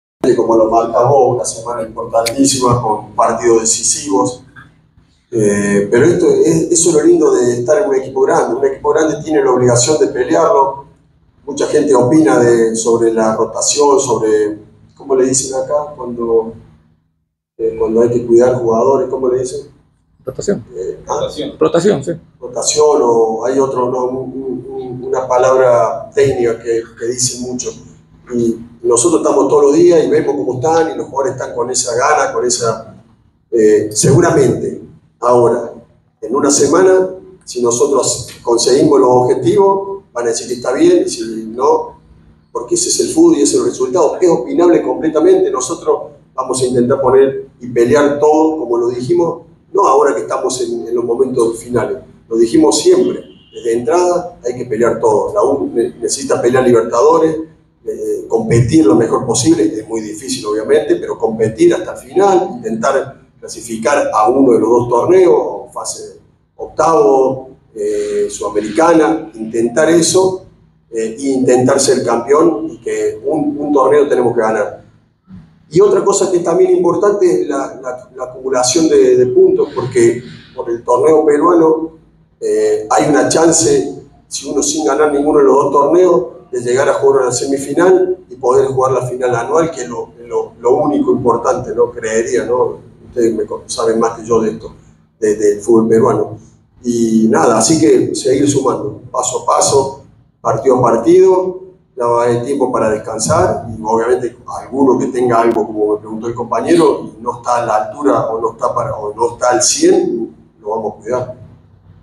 FABIAN BUSTOS "COMO EQUIPO GRANDE TENEMOS QUE PELEAR AMBOS FRENTES"
fabianbustos hablo tras el empate entre